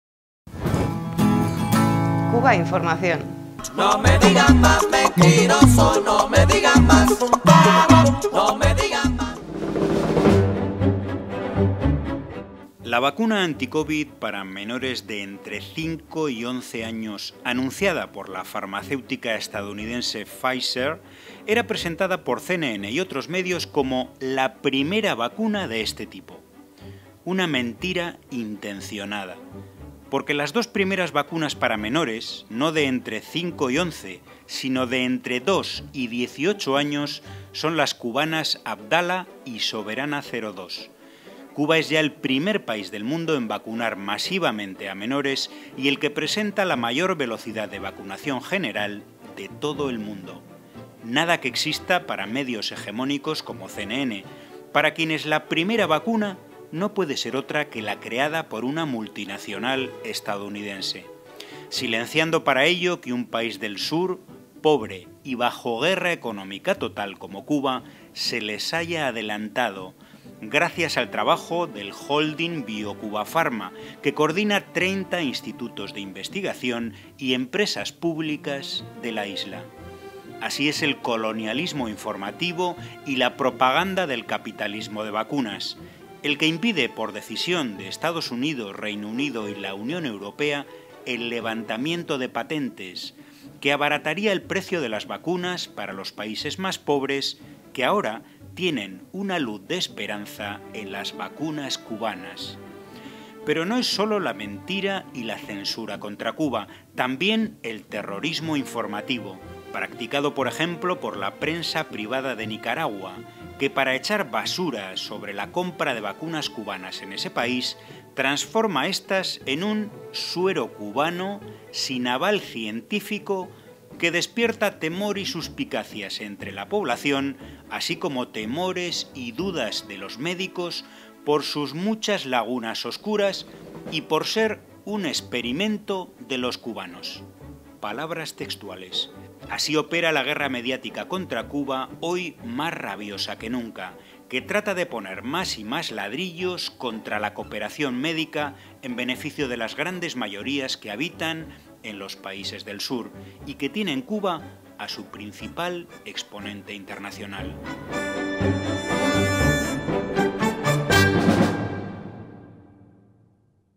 Primera vacuna anti-Covid para menores: ¿en EEUU… o en Cuba?
+Italiano/Français/ Deutsch/Inglés)

Primo vaccino anti-Covid per minori: negli USA... o a Cuba?
Premier vaccin anti-Covid pour les enfants : aux États-Unis... ou à Cuba ?
Der erste Impfstoff anti-Covid für Minderjährige: in den USA ...oder in Kuba?
First anti-Covid vaccine for minors: in the US ... or in Cuba?

La vacuna anti-Covid para menores de entre 5 y 11 años, anunciada por la farmacéutica estadounidense Pfizer, era presentada por CNN y otros medios como “la primera vacuna” de este tipo. Una mentira intencionada.